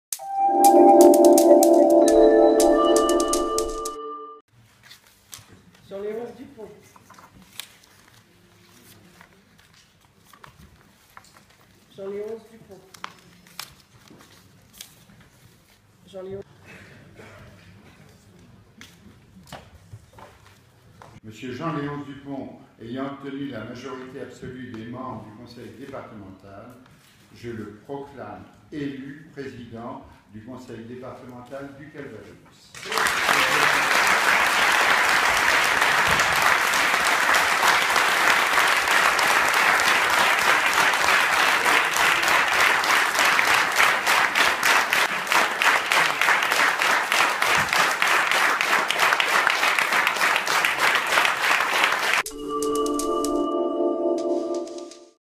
Élection de Jean-Léonce Dupont, président du conseil départemental du Calvados
Elections